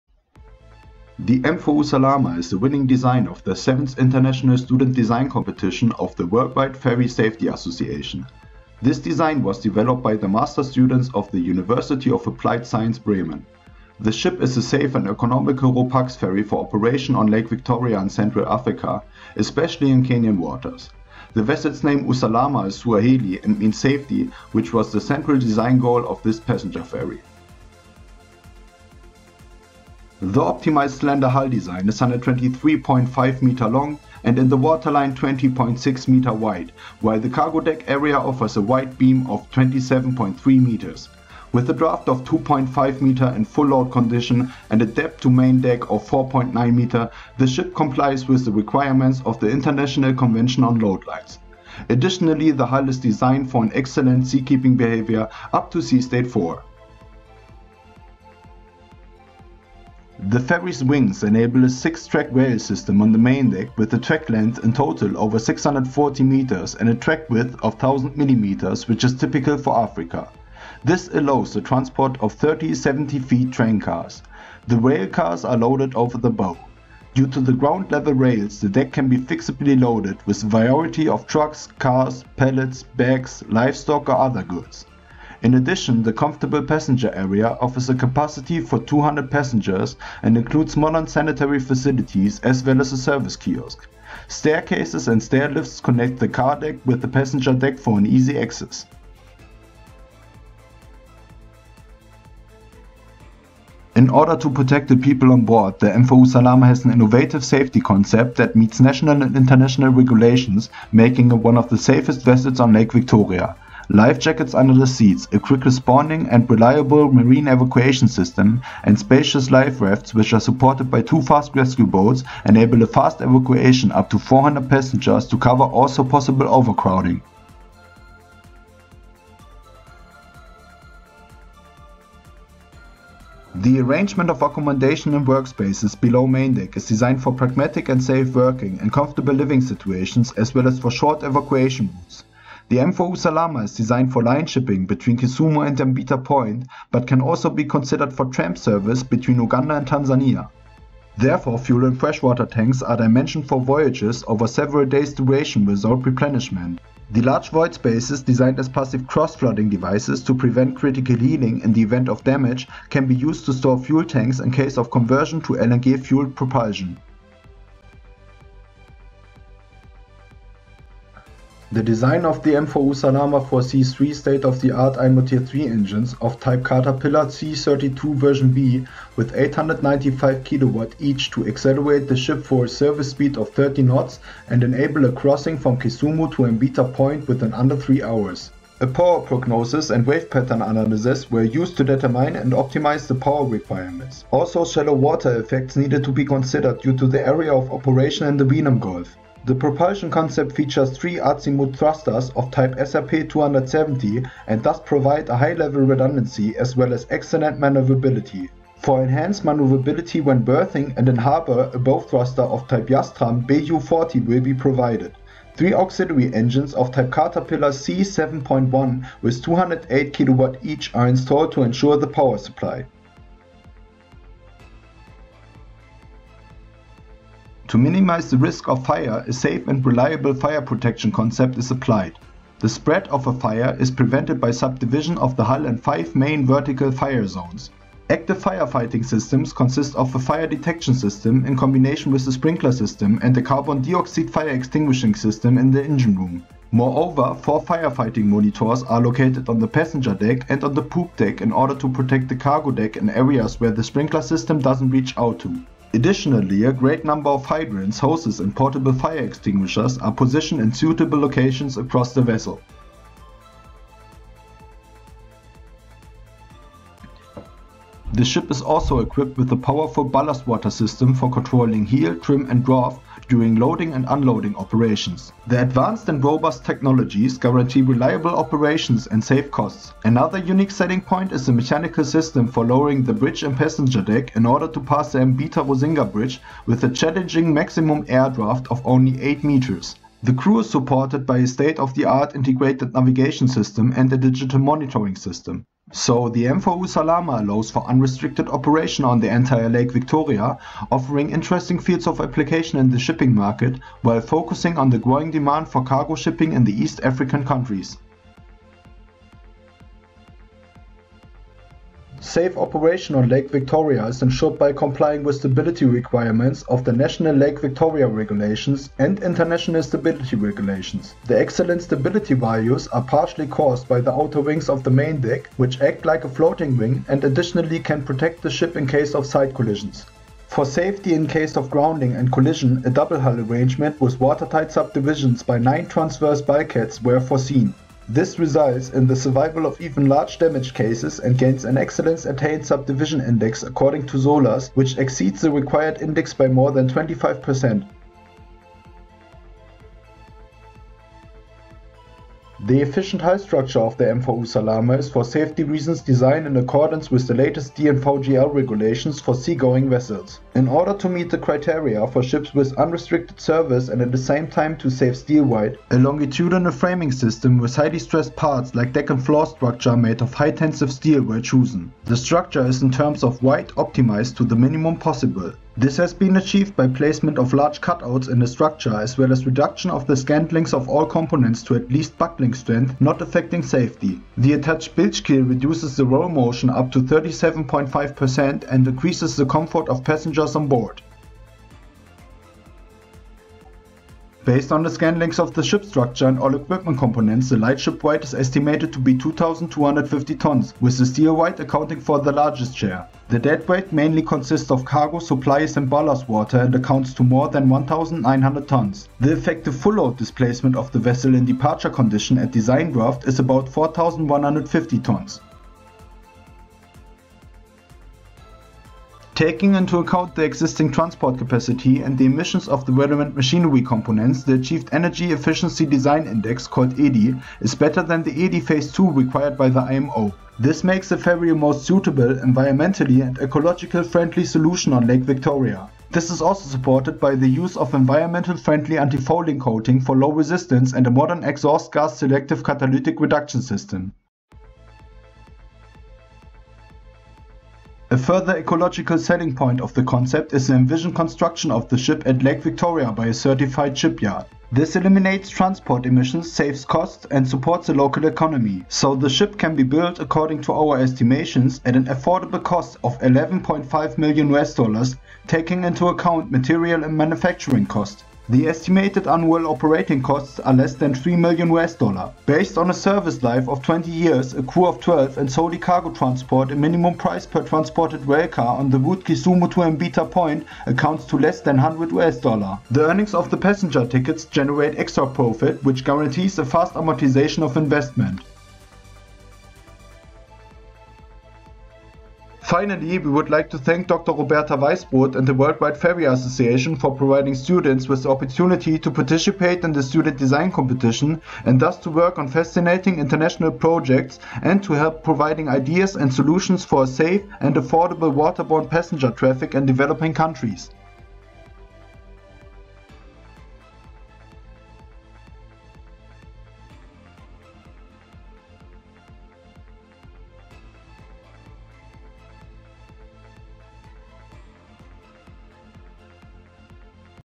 Bremen Hochschule team - 1st prize 2020
This video was produced by the first prize winning team from the Hochschule Bremen City University of Applied Sciences to describe their design for a ferry for carrying passengers and freight on Lake Victoria, MV Usalama.